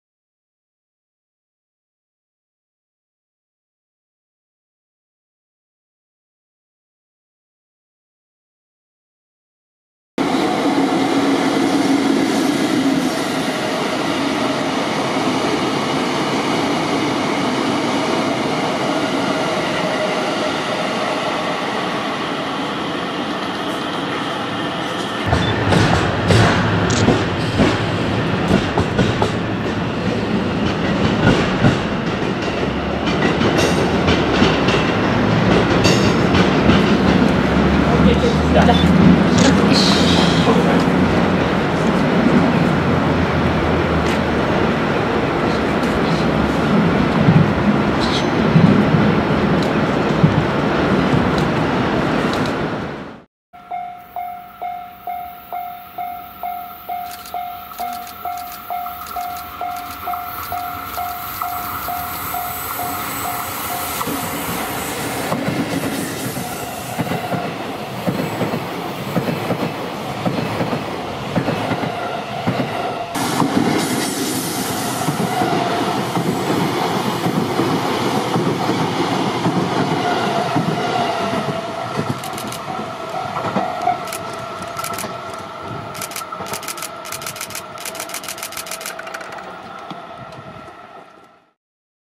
つくばエクスプレス3000系甲種輸送列車 TsukubaExpress new train 3000 series transport
つくばエクスプレスの増備車両である3000系第2編成が甲種輸送されました
2019/12/15 東海道線静岡駅、草薙～清水（静鉄狐ヶ崎付近）にて撮影

Tsukuba Express 3000 series
Transport new cars
2019/12/15 Shizuoka Station, Tokaido Line, Kusanagi-Shimizu
(Near Shizutetsu Kitsunegasaki station)